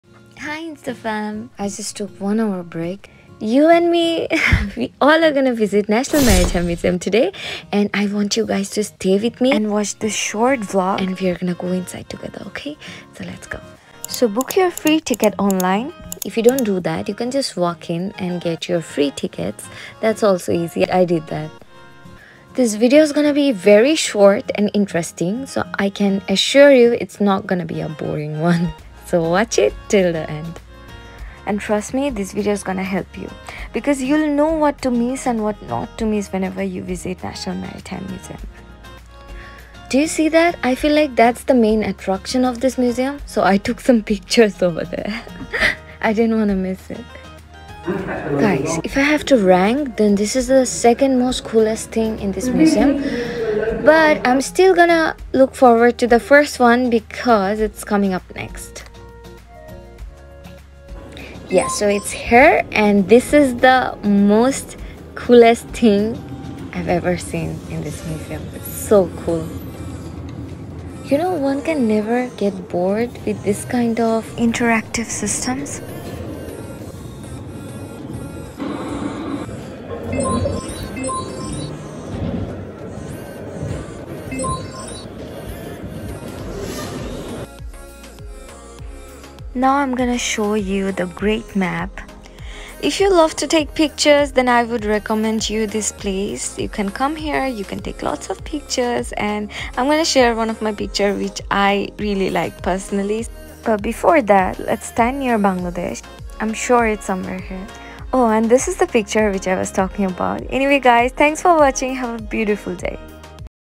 MY VISIT TO NATIONAL MARITIME MUSEUM 🏛 GREENWICH | QUICK VISIT | Best things to see in london 2021
Hi, being a University of Greenwich student, everything is so near to me! Enjoy my vlog!
This is about my Quick visit my National Maritime museum! Comment if you have any queries, i’ll reply ❤️🏛